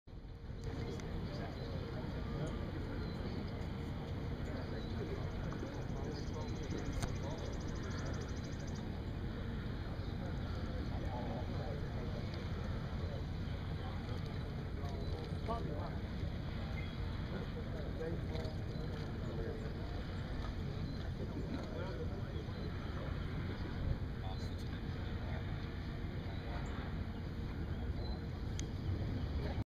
Augusta National Golf Course #15 Green view from right of fairway
15th at Augusta National (Masters Practice Round) Robert Garrigus waiting to hit 2nd shot on this par 5 hole